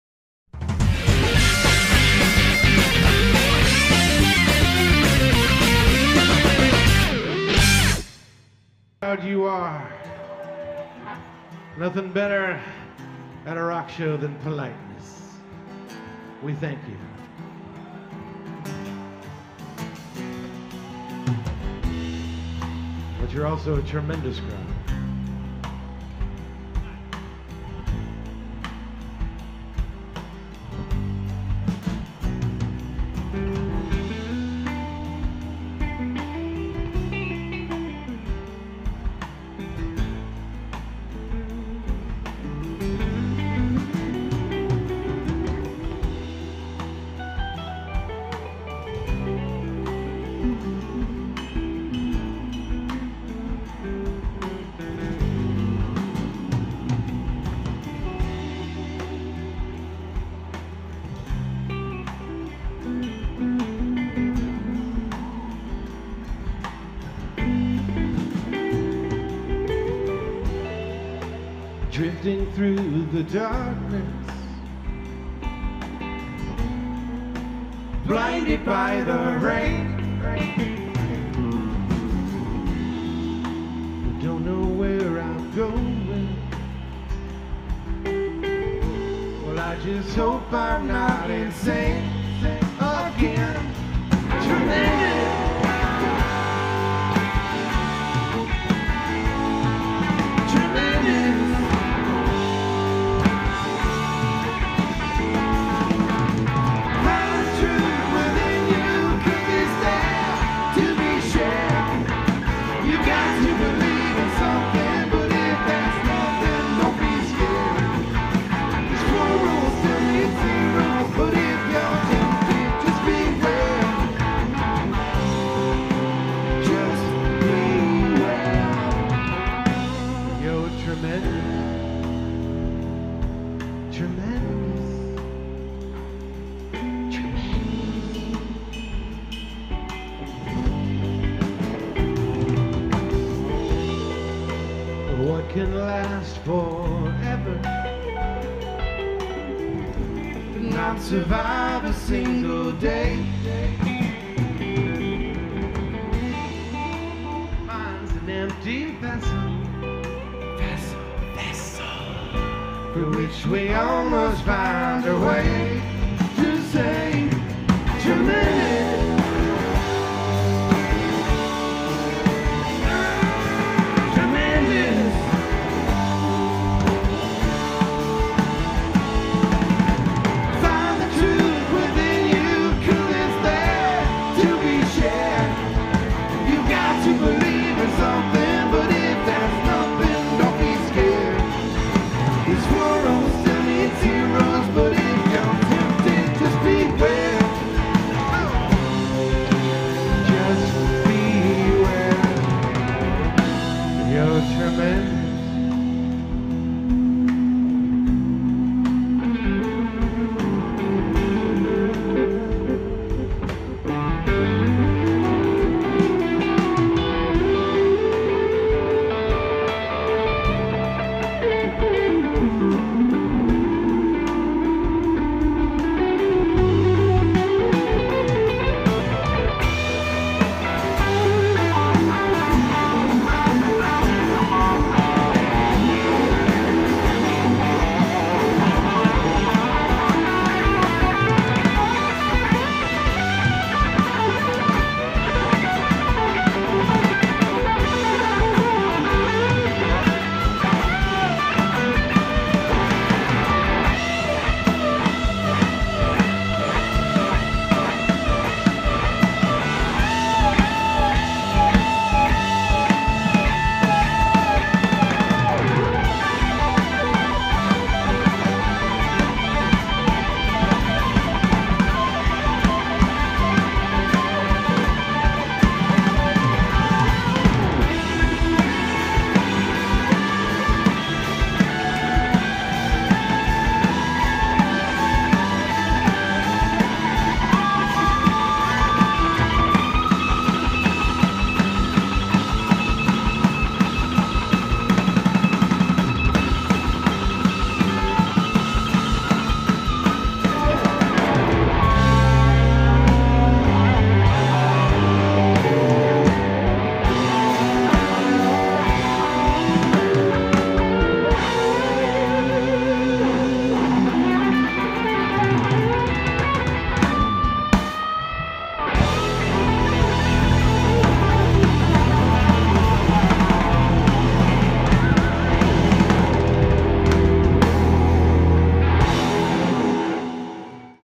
Kyle Gass Band op 42e Nirwana Tuinfeest (6)
De Kyle Gass Band trad op zaterdag 19 augustus op tijdens het 42e Nirwana Tuinfeest in Lierop.